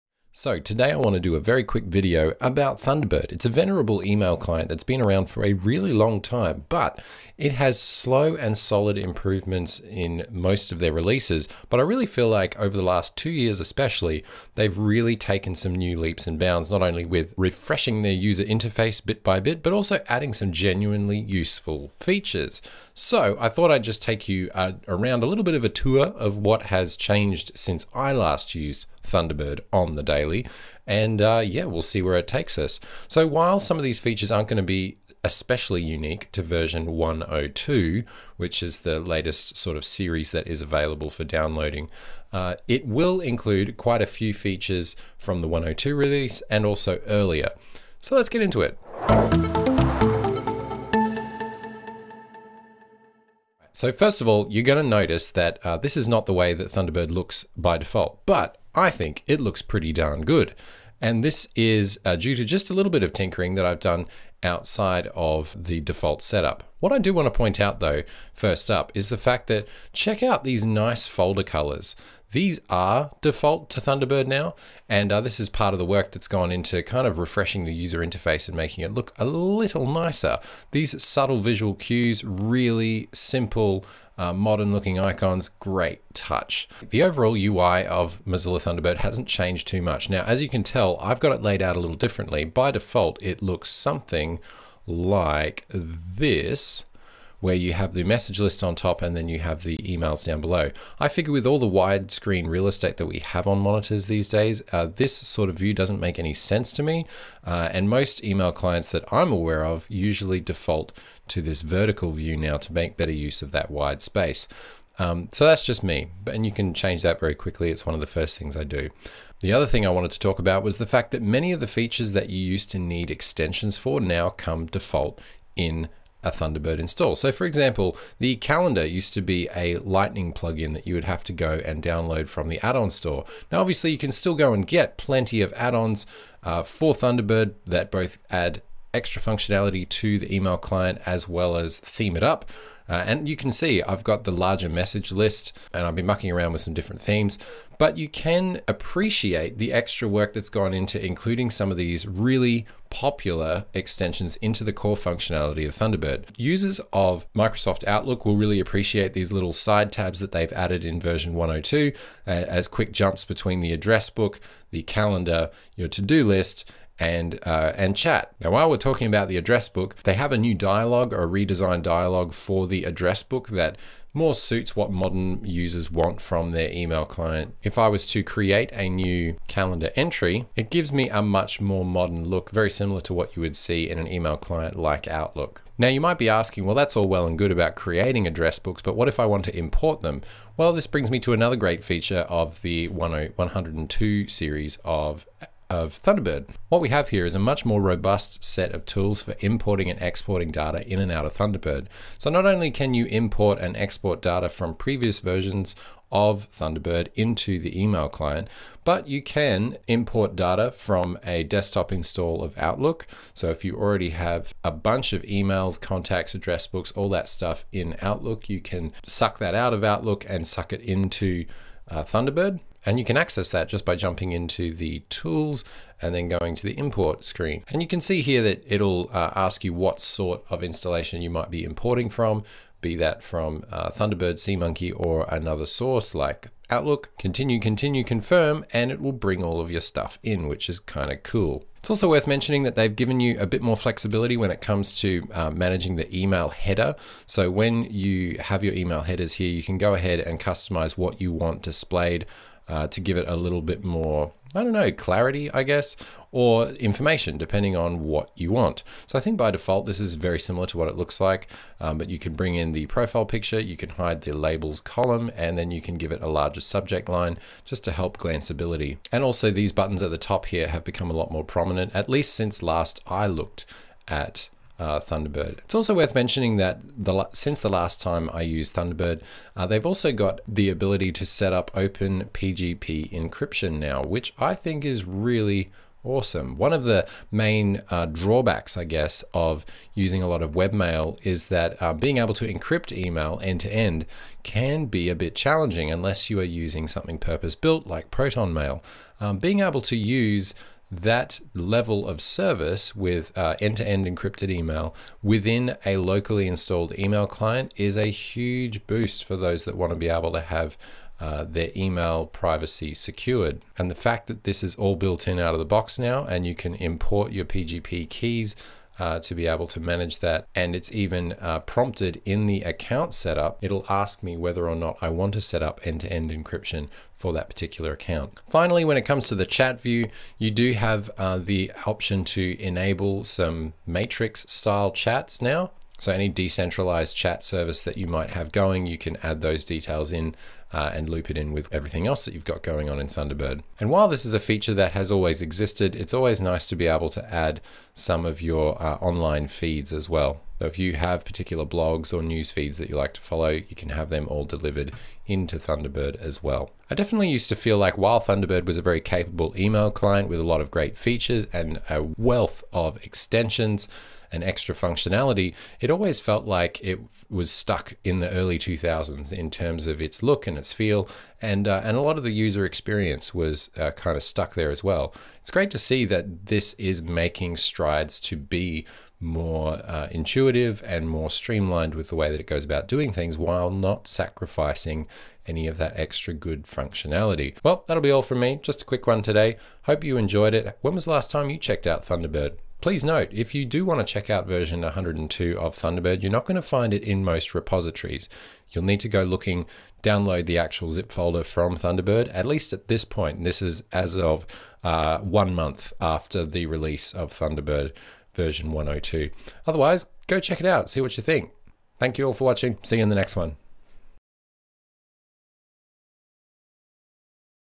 Thunderbird ISN'T from 2003 anymore!
Time to have a quick look through the latest version of the venerable Thunderbird email client - lots of features - but is it easier to look at and use?